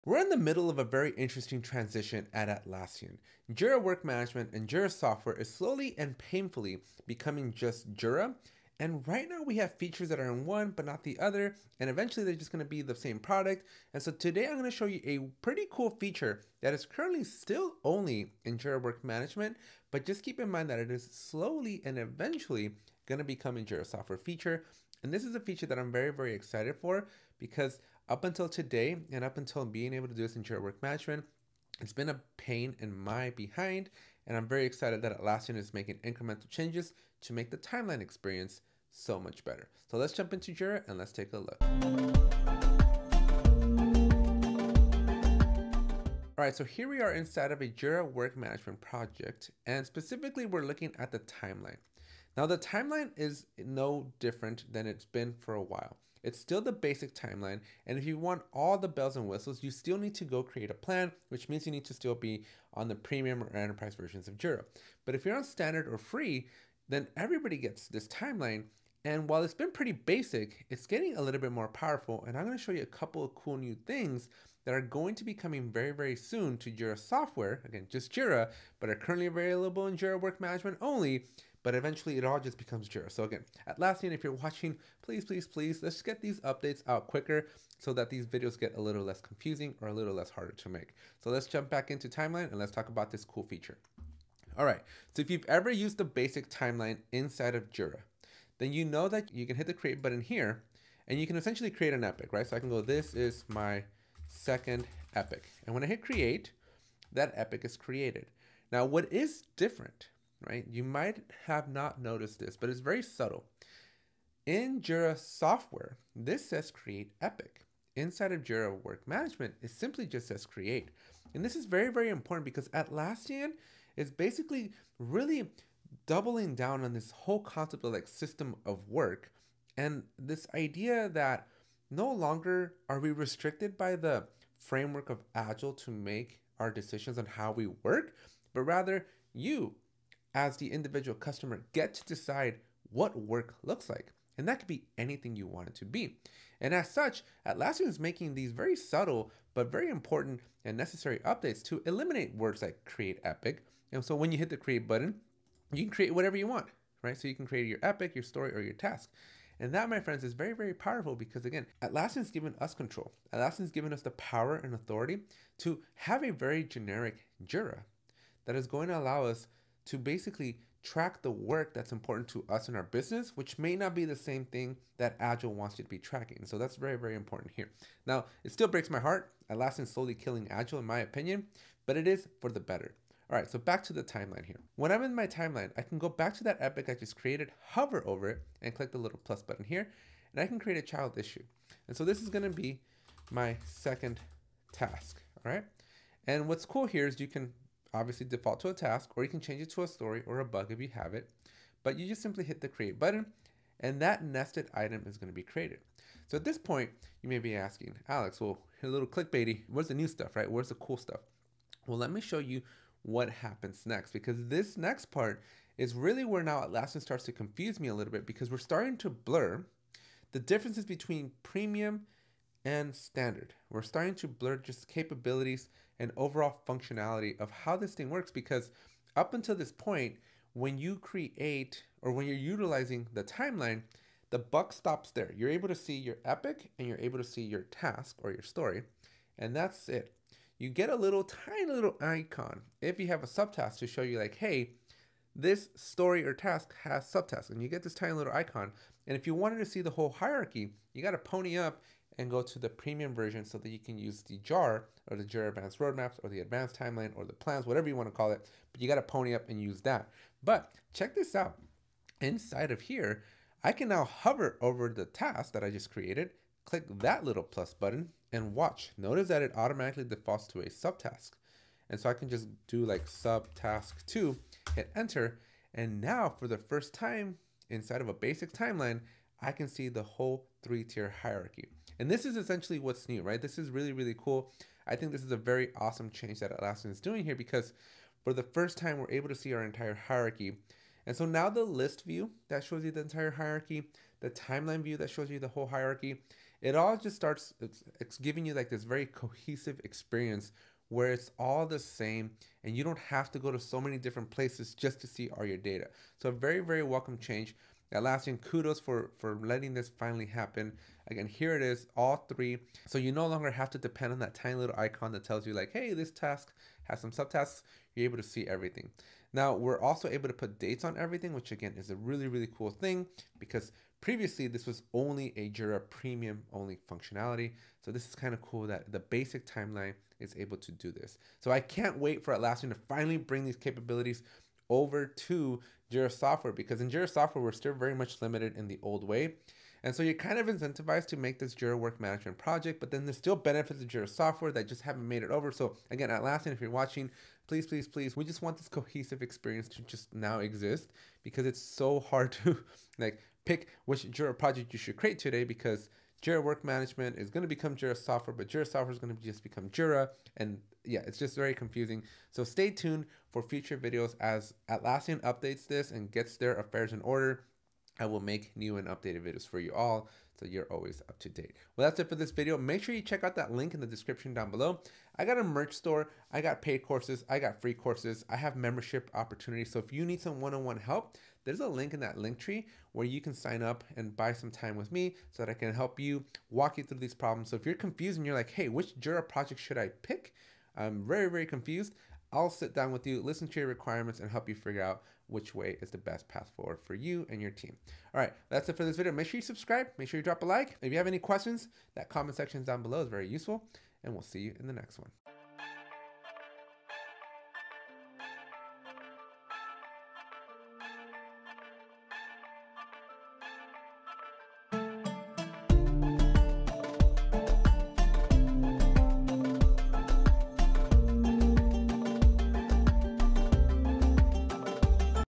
The BEST Jira Timeline Updates You Need to Know About
Learn about the basics of timeline updates in Jira. This video will walk you through everything you need to know about managing timelines in Jira! Perfect for beginners.